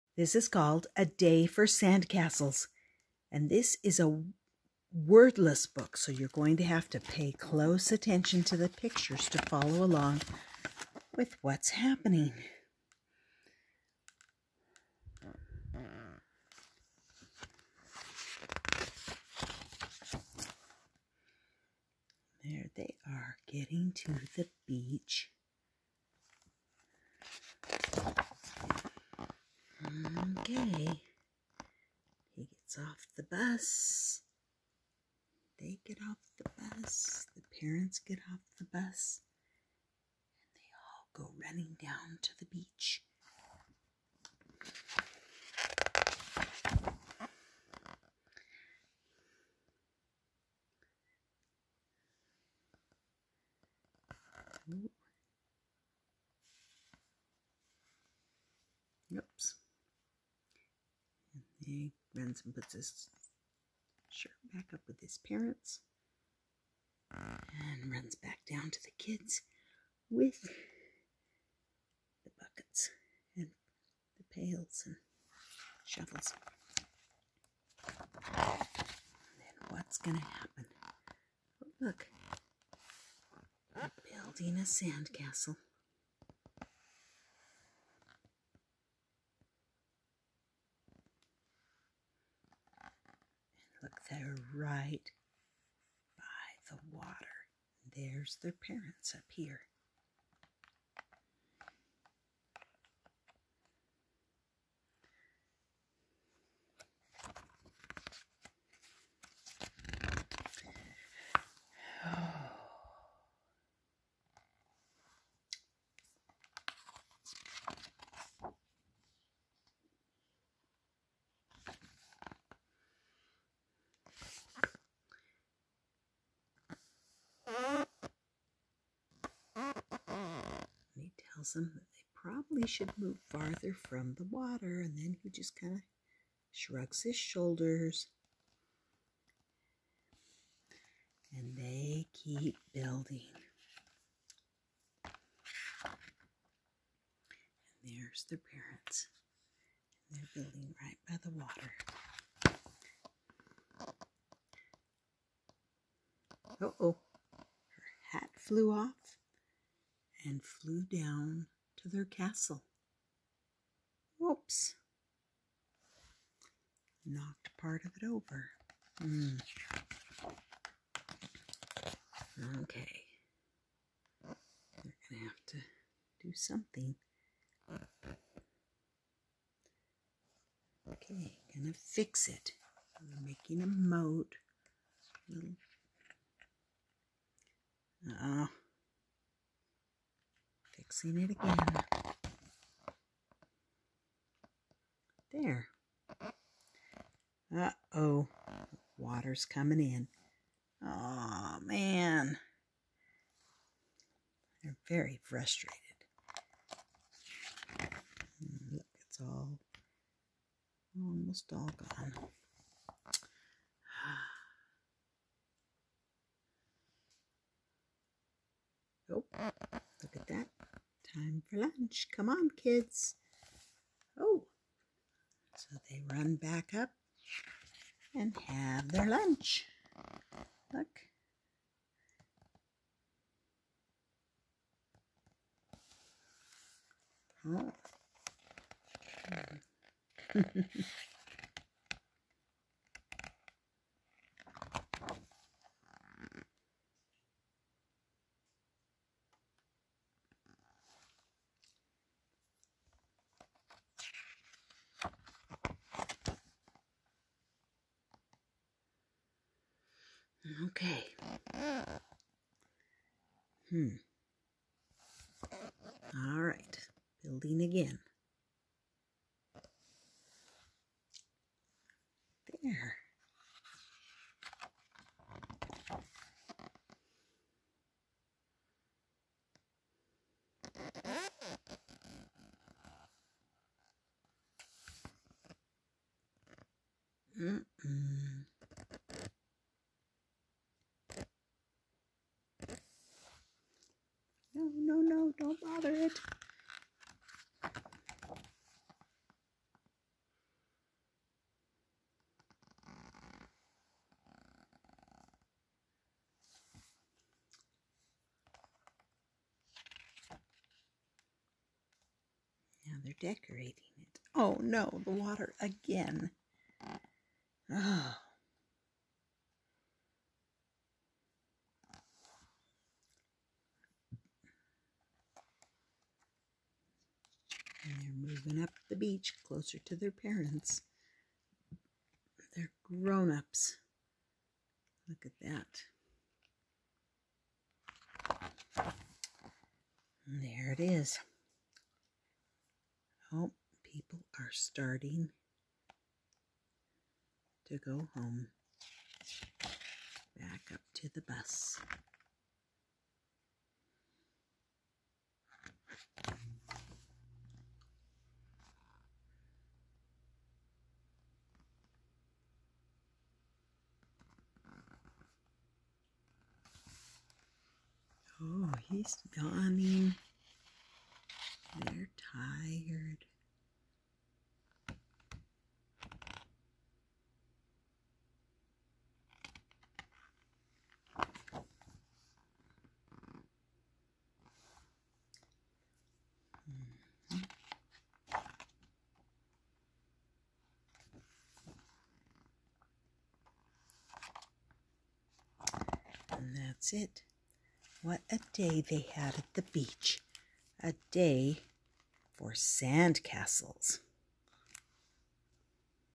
A Day For Sandcastles
It's a lovely summer day, perfect for spending at the beach. A busload of beachgoers disembark and set up along the sand to enjoy the cool breezes and sun. A trio of siblings decide they will spend their day in the best possible way: building a sandcastle. They pat and shape the sand as the sun travels across the sky. With each sandcastle iteration, they are met with some unforeseen obstacle: a hat gets caught in the wind and topples their castle, a toddler walks through it, and the tide continues to creep ever closer. But with each demolition, the determined builders find ways to construct a new sandcastle, bigger and better than the one that came before, until it is time to load back into the bus and head home.

Written by JonArno Lawson.
Illustrated by Qin Leng.

Summer beach